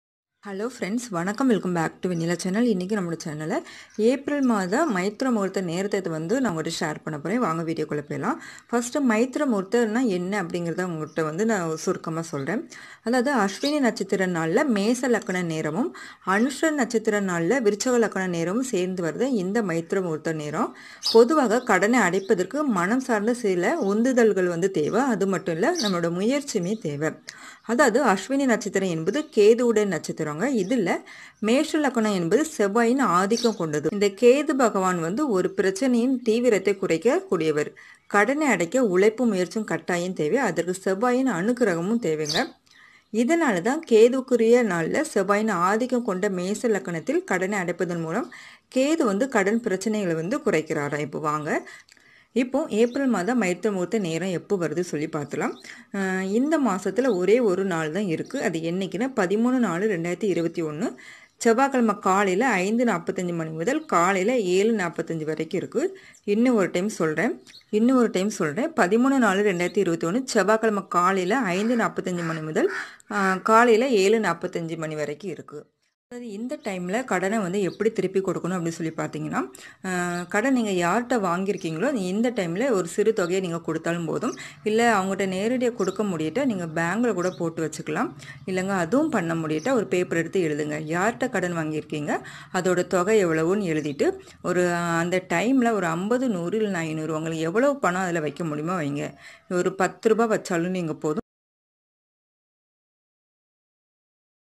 Maitreya muhurtham April 2021/ மைத்ர முகூர்த்தம் 2021 ஏப்ரல்/
மைத்ர முகூர்த்தம் என்றால் என்ன?

அசுவினி நட்சத்திர நாளில் மேஷ லக்ன நேரமும் அனுஷ நட்சத்திர நாளின் விருச்சிக லக்ன நேரமும் `மைத்ர முகூர்த்தம்' எனப்படுகின்றன.

பொதுவாகவே கடனை அடைப்பதற்கு மனம் சார்ந்த சில உந்துதல்கள், முயற்சிகள் தேவை. அசுவினி நட்சத்திரம் என்பது கேதுவுடைய நட்சத்திரம். இதில் மேஷ லக்னம் என்பது செவ்வாயின் ஆதிக்கம் கொண்டது. கேது பகவான் ஒரு பிரச்னையின் தீவிரத்தைக் குறைக்கக் கூடியவர். கடனை அடைக்க, உழைப்பும் முயற்சியும் தேவை. அதற்குச் செவ்வாயின் அனுக்கிரகம் தேவை.

எனவேதான் கேதுவுக்கு உரிய நாளில் செவ்வாயின் ஆதிக்கம் கொண்ட மேஷ லக்ன காலத்தில் கடனை அடைப்பதன் மூலம் கேது கடன் பிரச்னைகளைக் குறைத்து அருள்புரிவார்.

.Maitreya muhurtham  April 2021

நாட்கள் கடனை அடைக்க உகந்த கிழமை நேரம்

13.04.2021 செவ்வாய்க்கிழமை காலை 05.45 மணி முதல் காலை 07.45 வரை